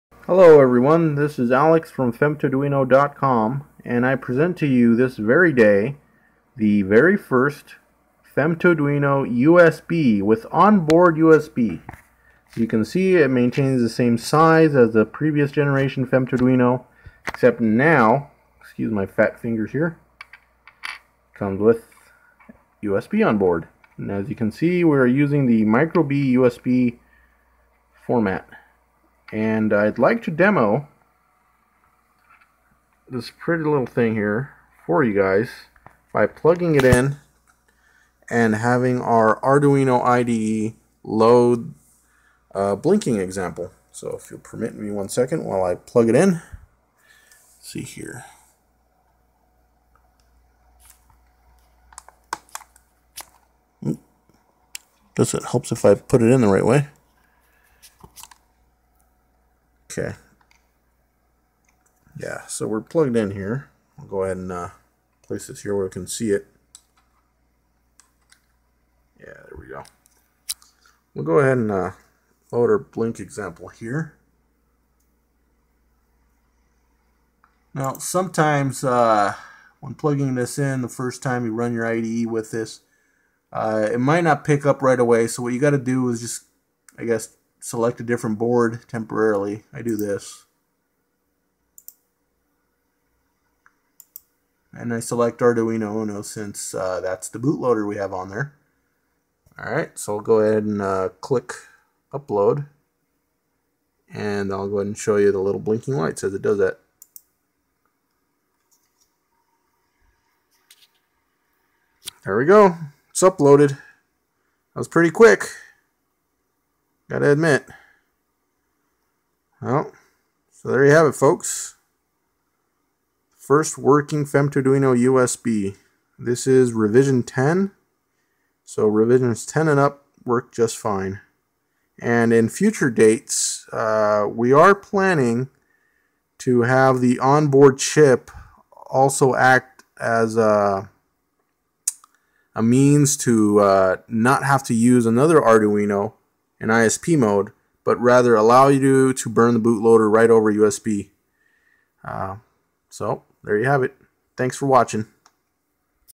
femtoduino with USB - smallest arduino clone
Introducing the femtoduinoUSB, the smallest arduino, now with USB! Also available on tindie.com April 20, 2013 and onward.